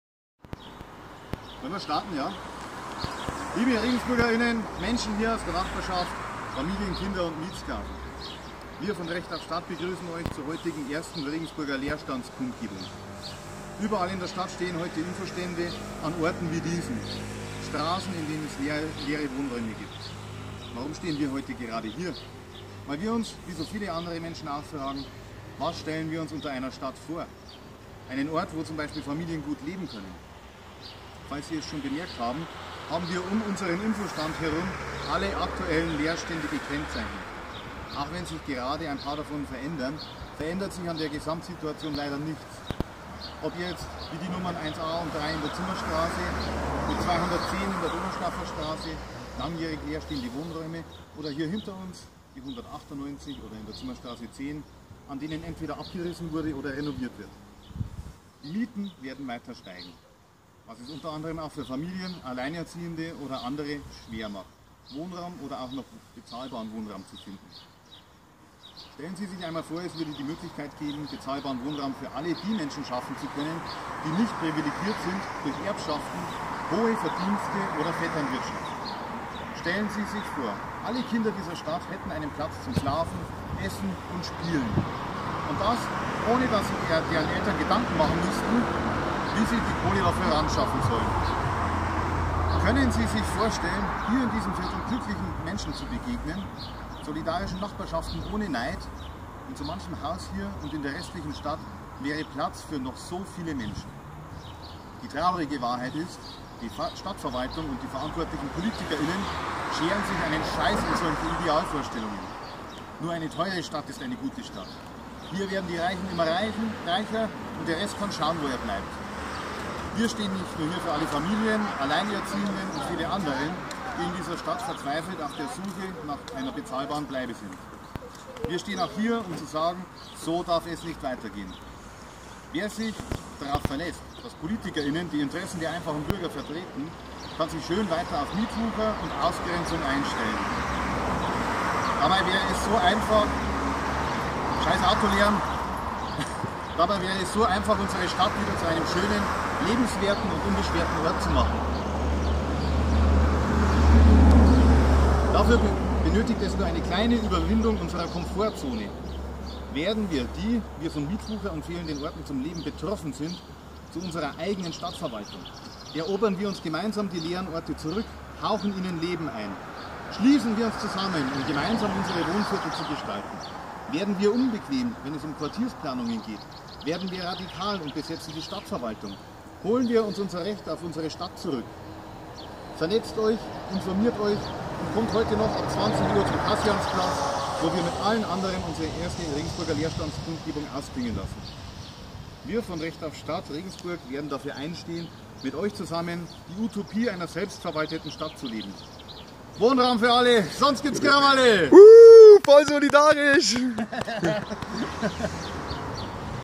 Rede von Recht auf Stadt – 1. Leerstandsaktionstag 10.07.2021
Am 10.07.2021 veranstaltete Recht auf Stadt zusammen mit anderen Gruppen den 1. Leerstandsaktionstag in Regensburg. Hier die Rede von unserem Kundgebungsort in der Ecke Donaustauferstraße / Zimmerstraße. Dort stehen Häuser mit Gärten leer. In der Regel werden sie irgendwann abgerissen und durch Neubauten von unbezahlbaren Eigentumswohnungen ersetzt.